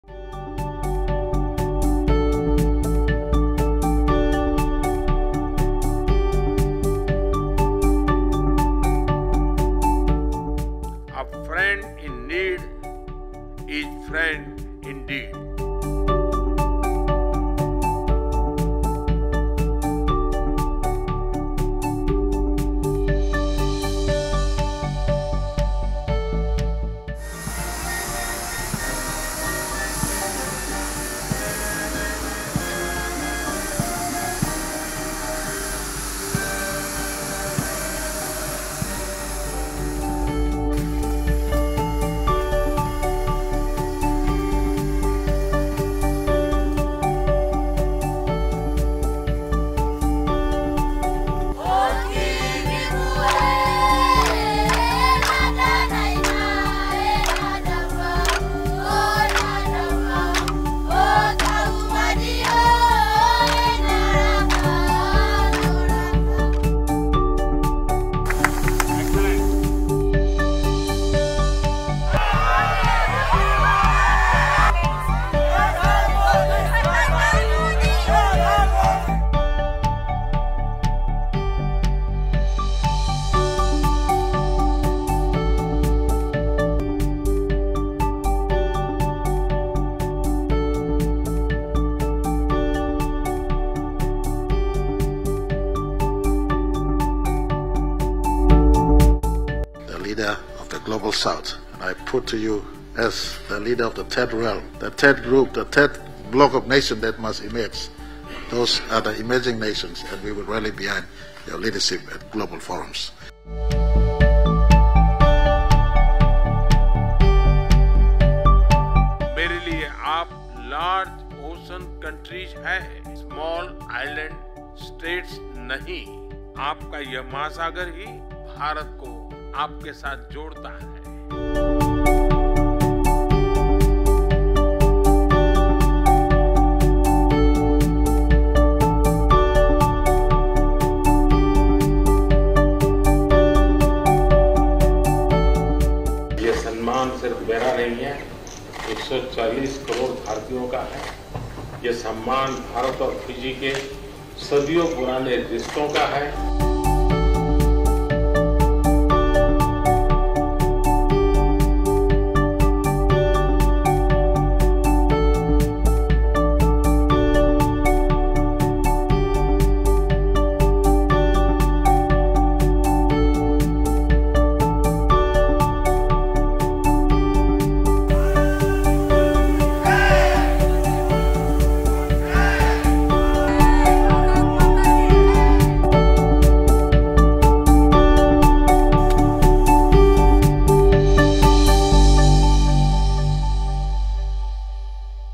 Relive the moments from PM Modi's LANDMARK visit to Papua New Guinea
Relive the moments from Prime Minister Narendra Modi's LANDMARK visit to Papua New Guinea.